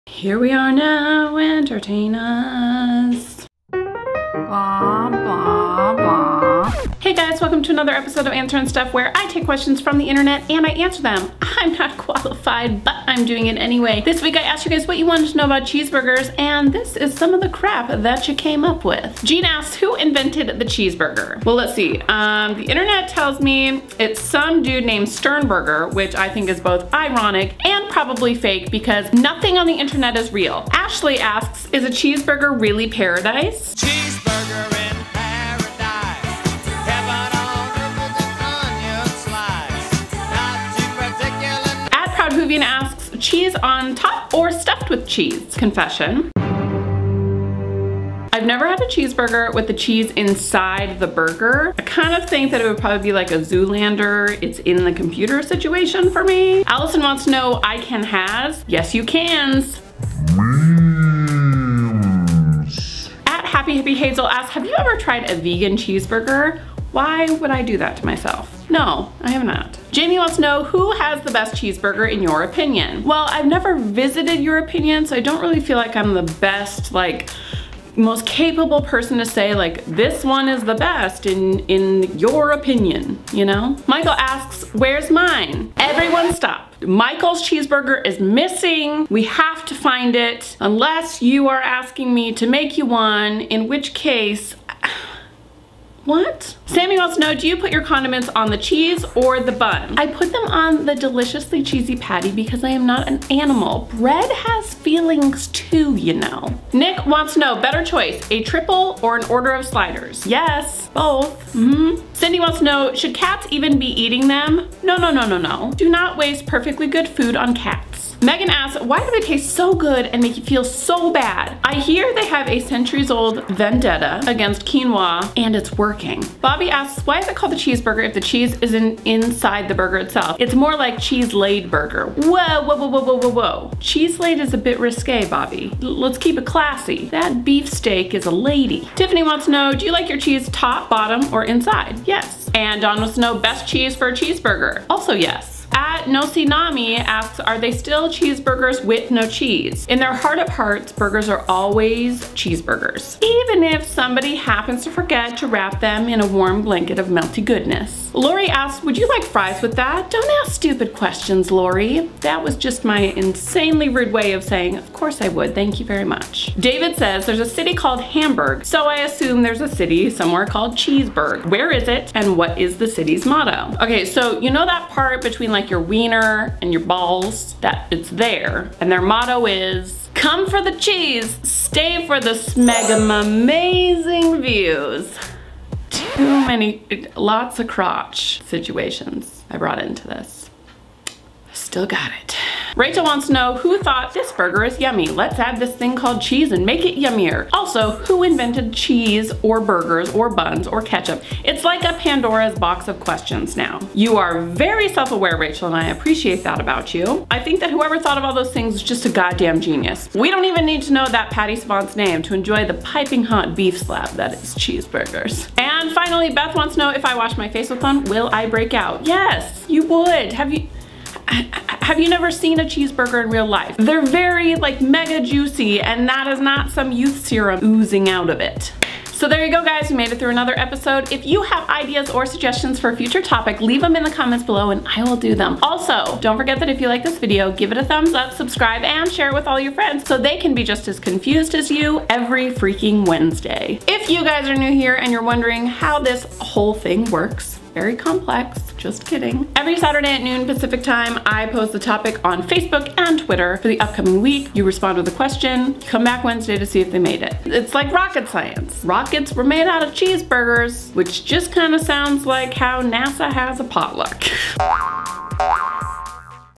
MY CHEESEBURGER CONFESSION | AnswerinStuff | SHUGGILIPPO
This week I asked you guys what you wanted to know about cheeseburgers on Facebook & Twitter and this is some of the crap that you came up with.

Hey! I'm Jess. I sometimes upload new videos on Mondays, Wednesdays & Fridays. Betcha feel good about this now.